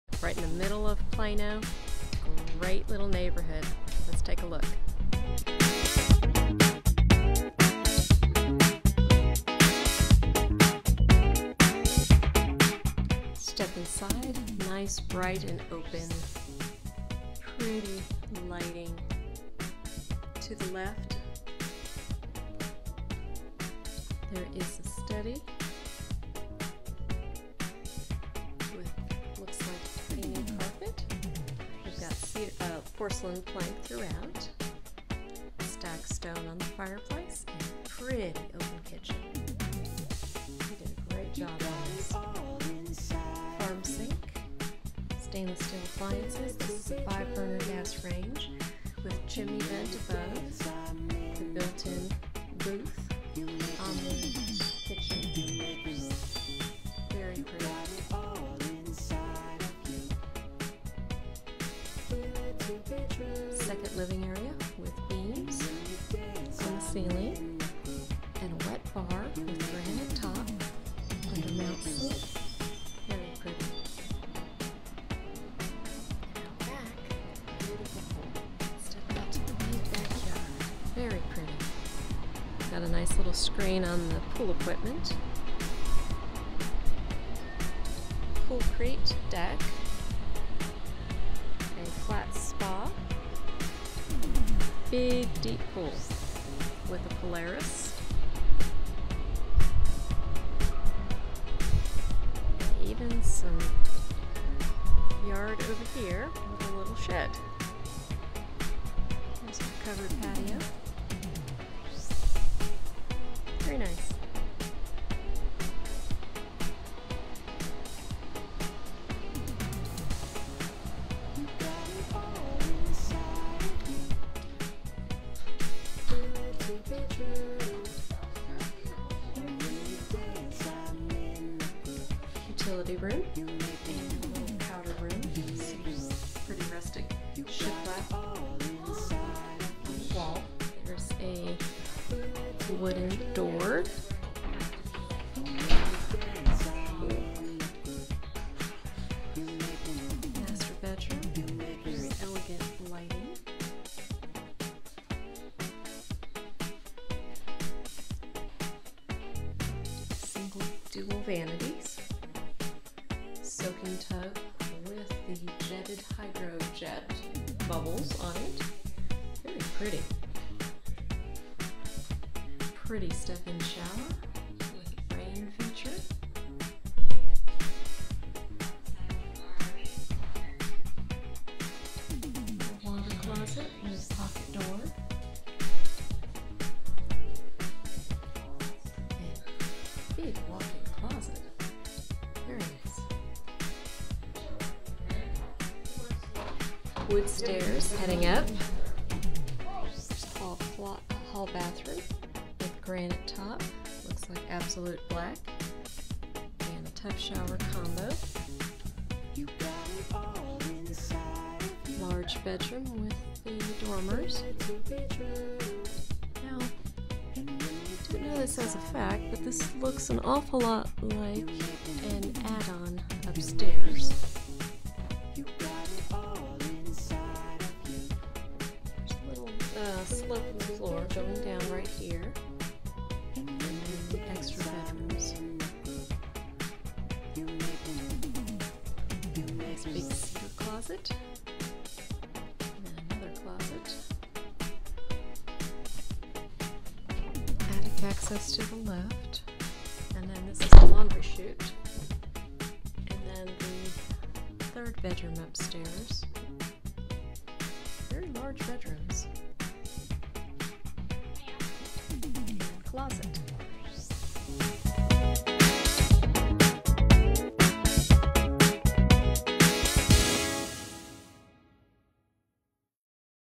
2809 Canyon Valley, Plano TX
Video tour of 2809 Canyon Valley, Plano TX by TX Realtor Kimberly Broussard. List price $549,000